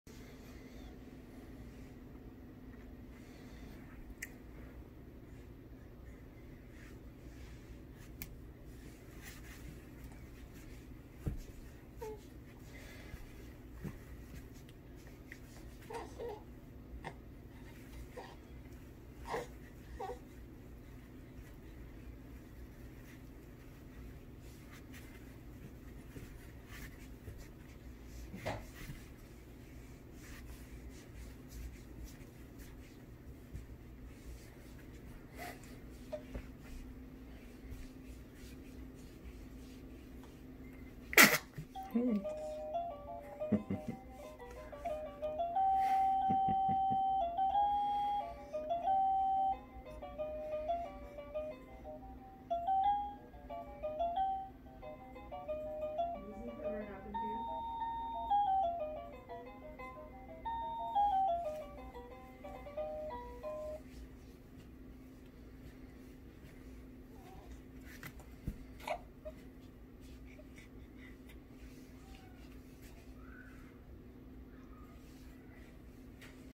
Introducing Baby Hope! - Super Cute - Baby sneeze turns on the music!
Baby Hope sneezes and activates her music box. Aww...